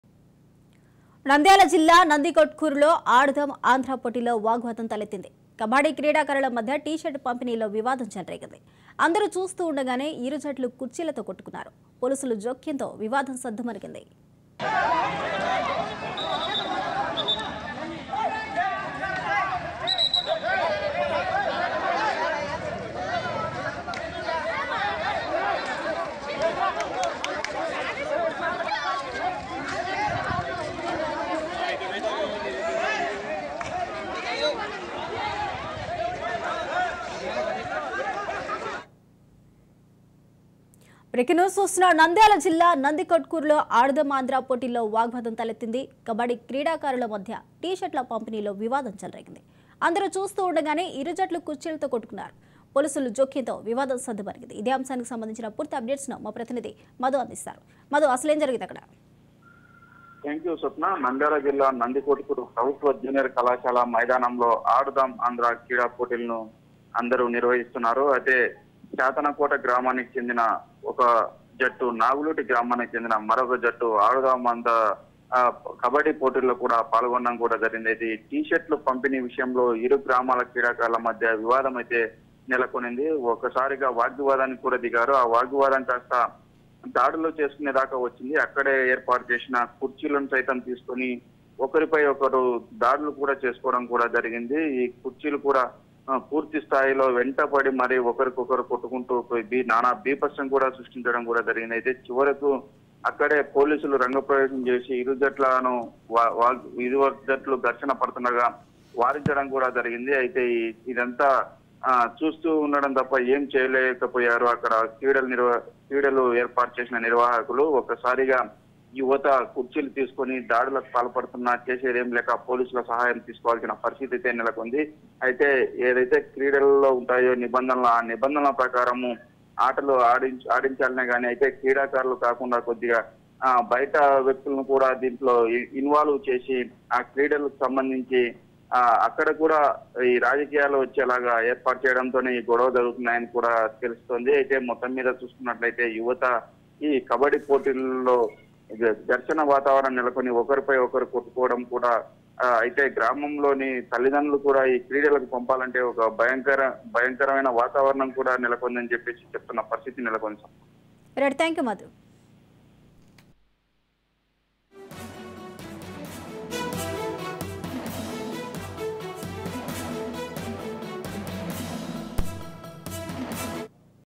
Clashes Between Players In Aadudam Andhra Tournament At Nandikotkur | Raj News Telugu
ఆడుదాం ఆంధ్రా పోటీల్లో ఆటగాళ్ల మధ్య ఘర్షణ | Clashes Between Players In Aadudam Andhra Tournament At Nandikotkur | Raj News Telugu